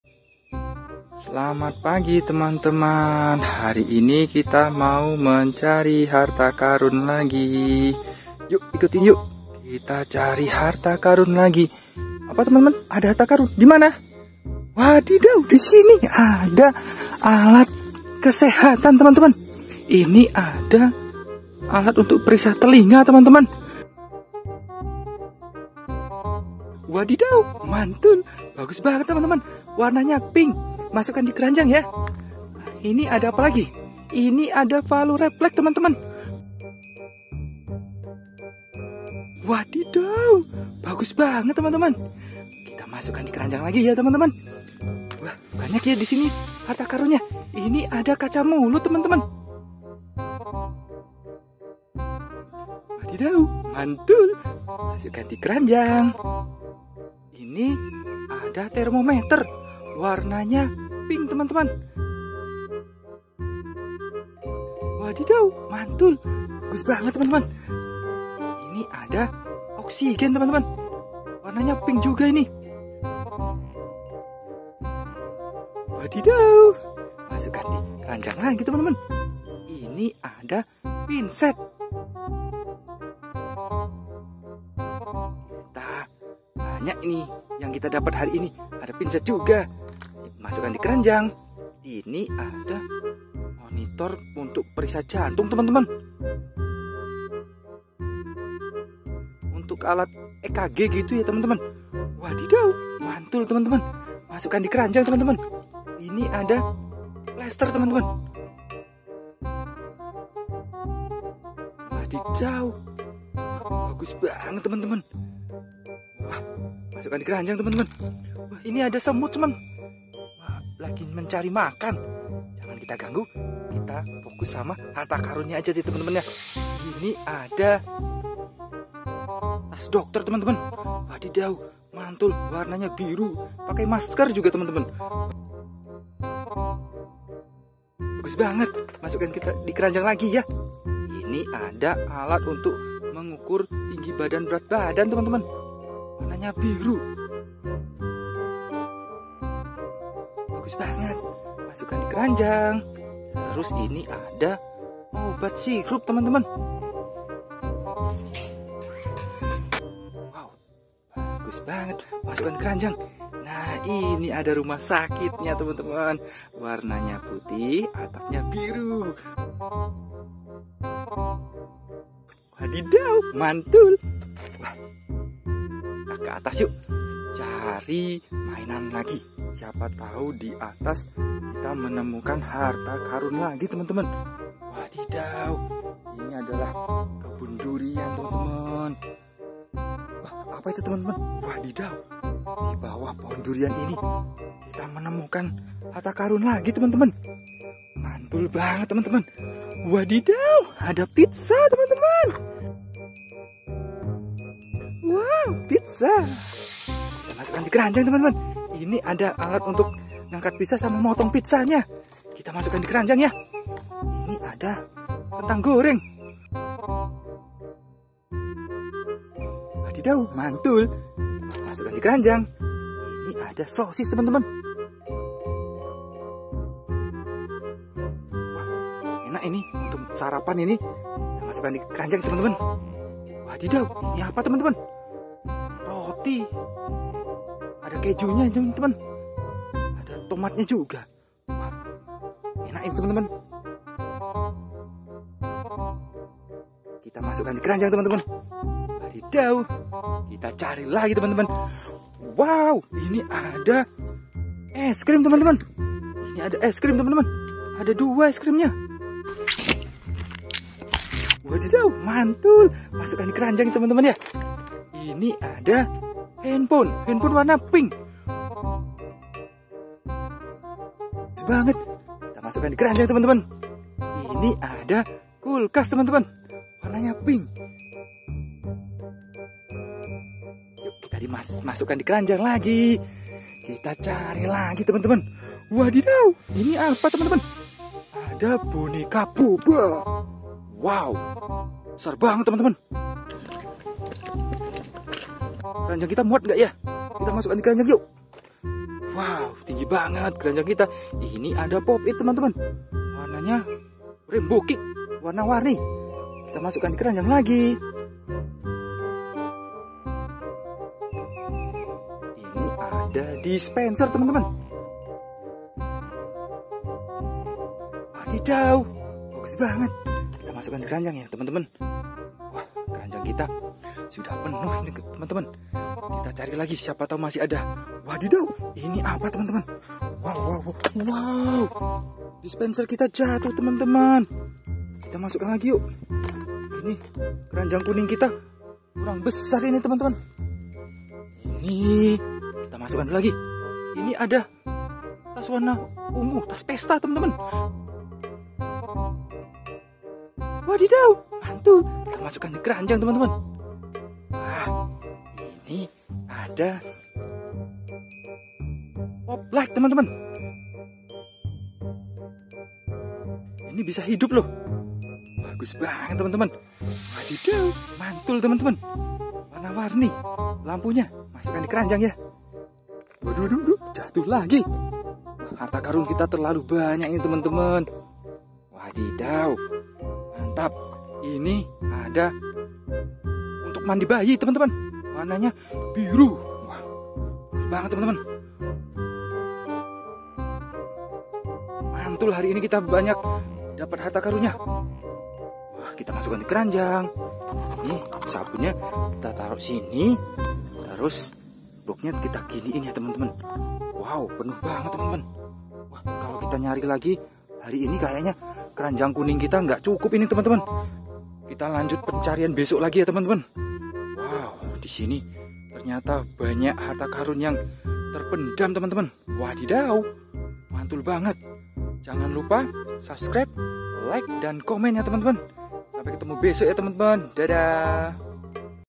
Hunting Stetoskop, Monitor Jantung, Oksigen, Termometer, Masker, Tas Pesta, Boneka Boba, Pizza
Yuck teman-teman kita Hunting Stetoskop, Oksigen, Plaster Luka, Termometer, Tas Pesta, Pop It, Boneka Boba, Pizza, Es Krim, dispenser, handphone, pop light, kulkas, dan alat-alat medis lainnya